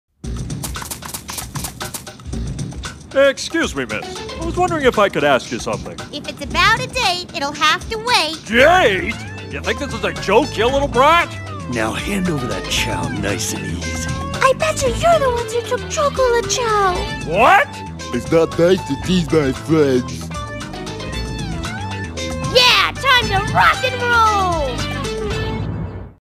if the Dingo Brothers came across Team Rose (read desc)
instead of Big The Cat being there, that Western Gray Kangaroo is Big's replacement, Western Gray Kangaroos are one of the species that the Dingo Brothers hate because they were the reason why the Australian Dingoes were going extinct